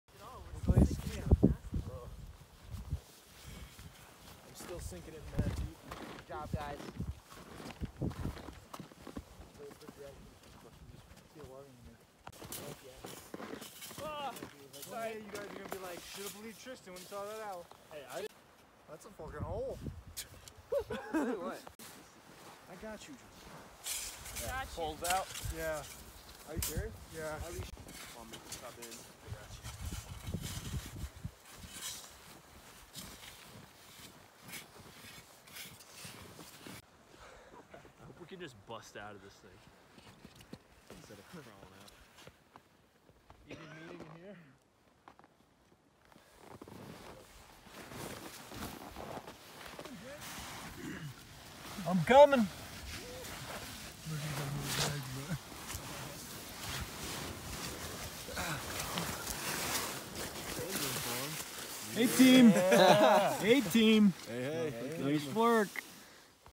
A Must Know for All Backcountry Users - Emergency Winter Ski Shelter
A demonstration of how to build an emergency Ski - Tarp - Trench Bivy Shelter. All you need is a shovel, tarp and your skis or poles. A simple solution to get you out of the elements fast!